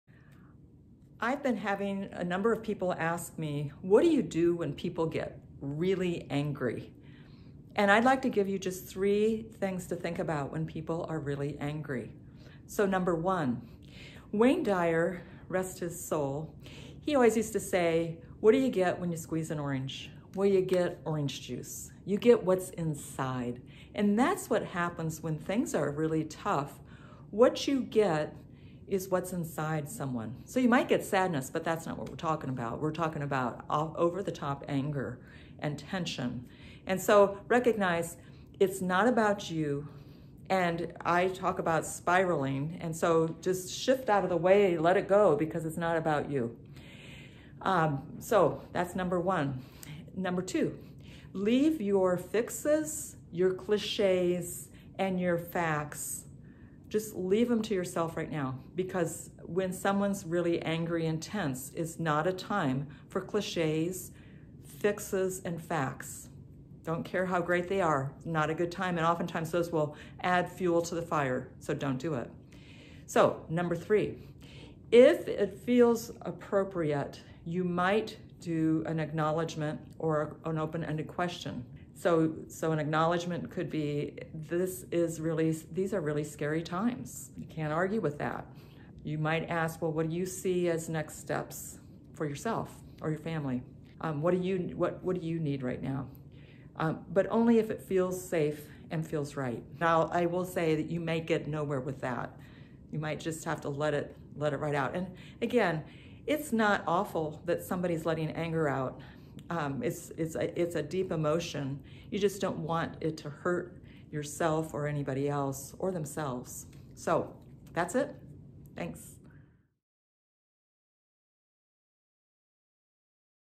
Three Tips to Deal with Other Peoples' Anger
In times of major disruption stress increases...what do you do when you find yourself in the presence of other peoples' anger.  Three tips!